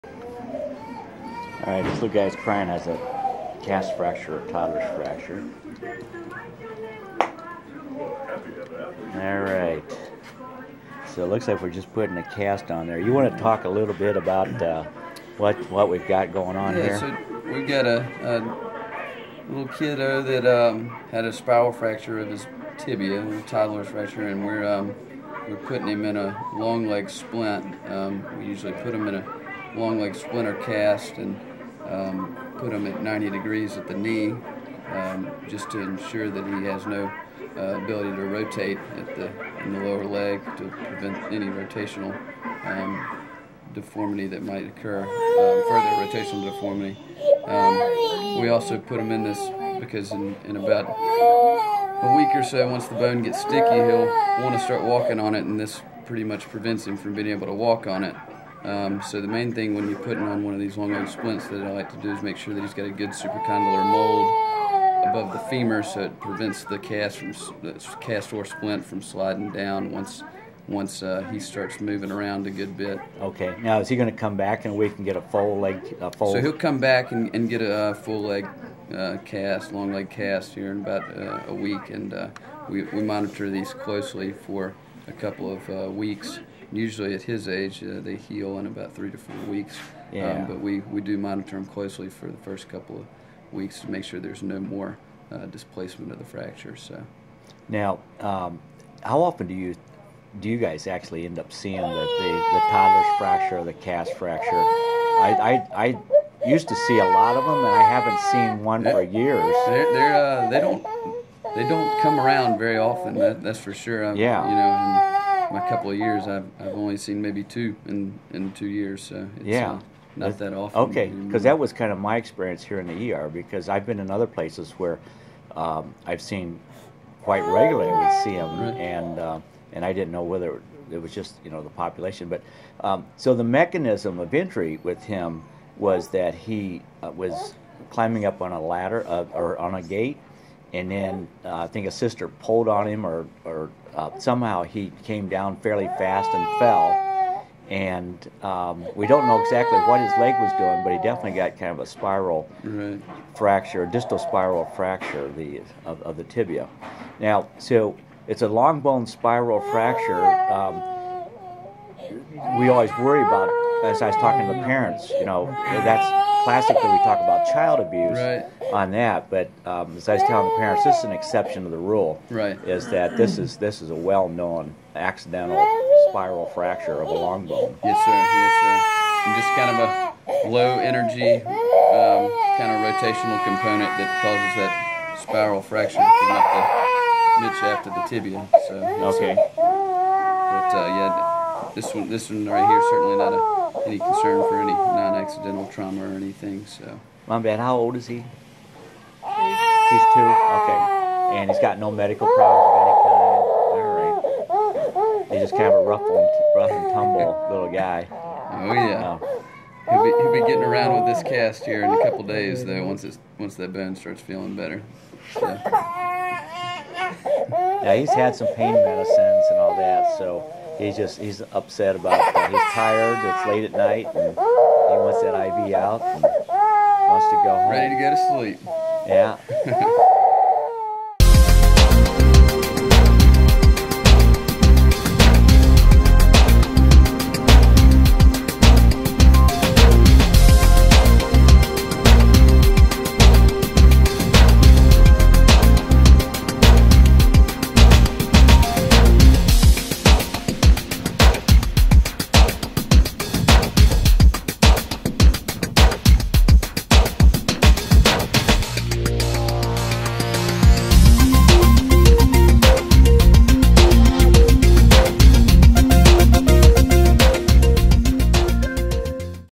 Childhood Accidental Spiral Tibial Fracture
The patient shown in this video has a Childhood Accidental Spiral Tibial (C.A.S.T.) Fracture that may also be called a "Toddler's Fracture".  I have had an interest in this specific fracture for several decades and actually suggested a name change based on the research I performed and published in 3 retrospective review papers.  The articles are now available on scribd. com.

This video was re-uploaded because of a technical problem with the first attempt.